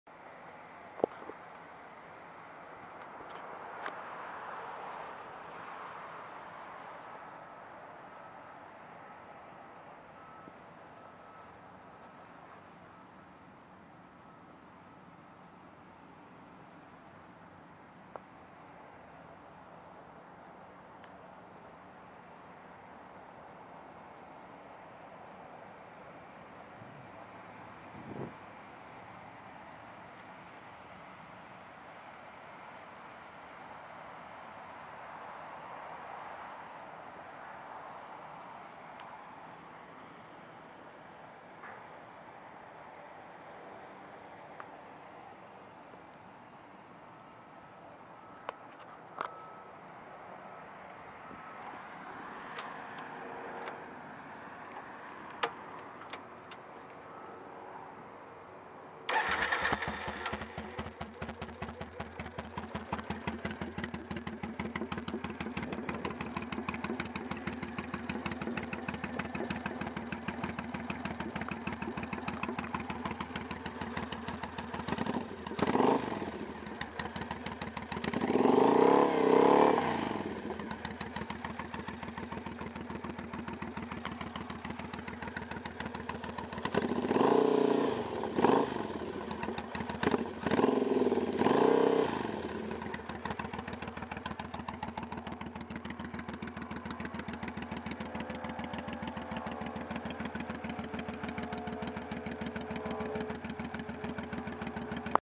2007 Triumph Speedmaster Build
2007 Triumph speedmaster, torn down to the fame. everything blacked out. paint, powdercoat and polishing